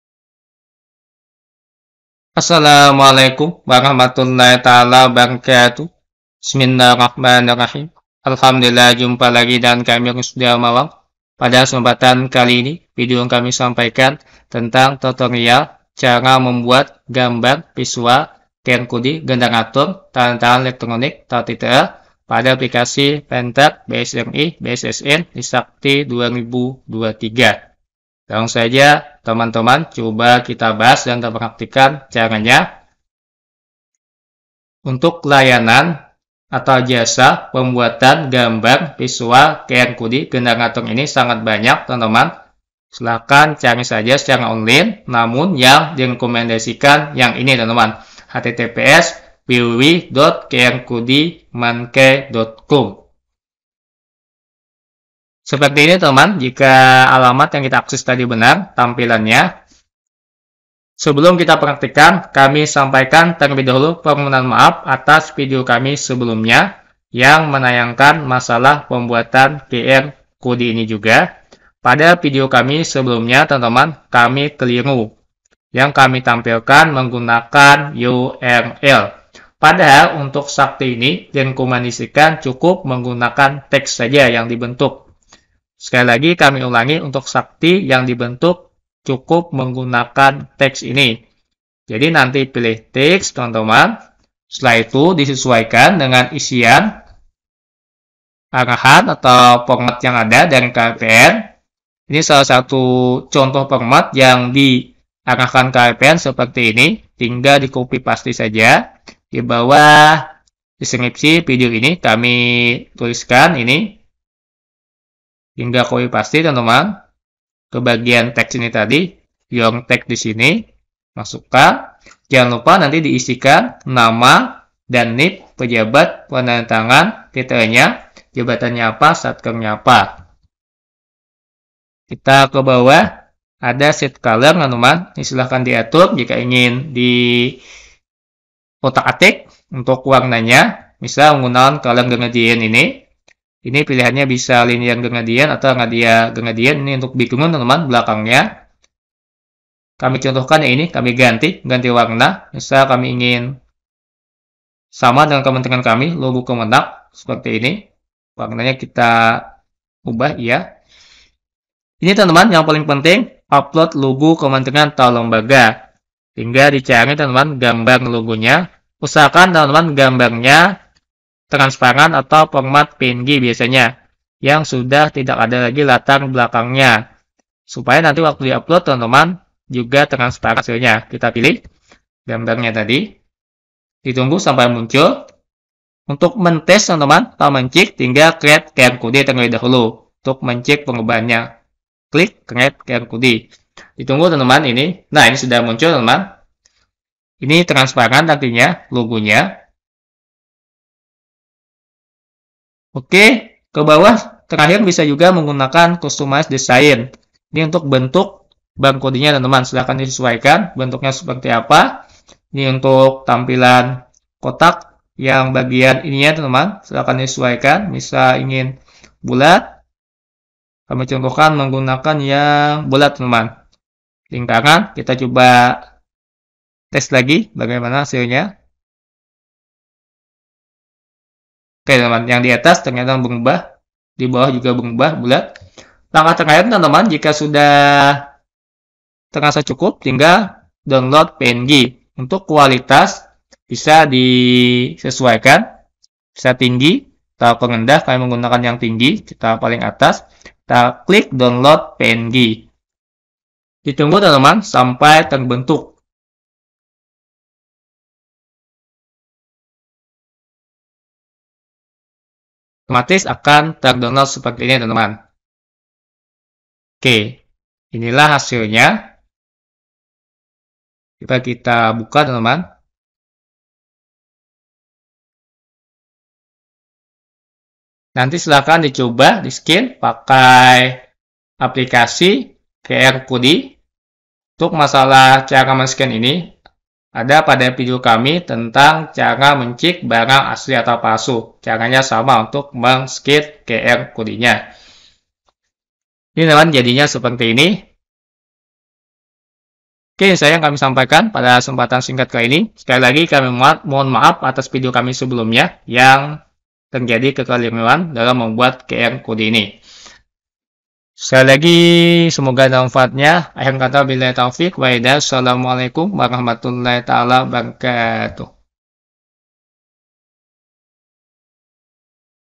Cara Membuat Gambar Visual QR Code Tanda Tangan Elektronik (TTE) Aplikasi Panter BSRE Di Sakti 2023
Cara Membuat Gambar Visual QR Code Generator Tanda Tangan Elektronik (TTE) Aplikasi Panter BSRE Di Sakti 2023
2. Pada Kolom Text Masukkan ini
Benar ini adalah dokumen yang saya tandatangani atas nama:
Nama :
NIP :
Jabatan : KPA/PPK
Satker : MTsN 1 Hulu Sungai Selatan
Kebenaran penyataan, perhitungan dan isi yang tertuang dalam dokumen ini menjadi tanggungjawab saya Semua bukti pendukung lampiran SPP & SPM telah diuji & dinyatakan memenuhi persyaratan untuk dibayar
3. Jangan Lupa Sediakan gambar logo Kementerian untuk diupload juga.
4. Setelah tinggal create QR Code tinggal download PNG
5. Selesai